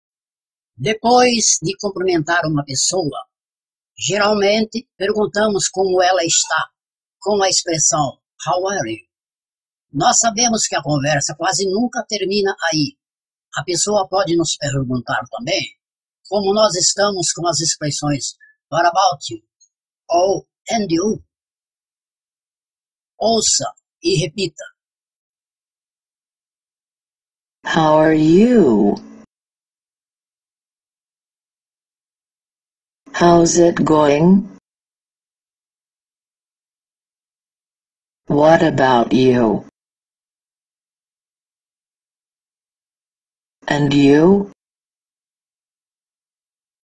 Curso Básico de Inglês para iniciantes-Lição 02-Greetings(Cumprimentos)
Learning English(Aprendendo inglês)Curso Básico de Inglês para iniciantes-Lição 02-Greetings(Cumprimentos)

Lição 02-Cumprimentos(Greetings)

Depois de cumprimentar uma pessoa, geralmente, perguntamos como ela está com a expressão: "How are you?". Nós sabemos que a conversa quase nunca termina aí, a pessoa pode nos perguntar também como nós estamos com as expressões "What about you?" ou "And you?".

Agora escute e repita três vezes:
  INGLÊS                                 PORTUGUÊS
 How are you?               Como você está?
 How's it going?               Como vai?
 What about you?            E sobre voce? E voce?
 And you?                        E você?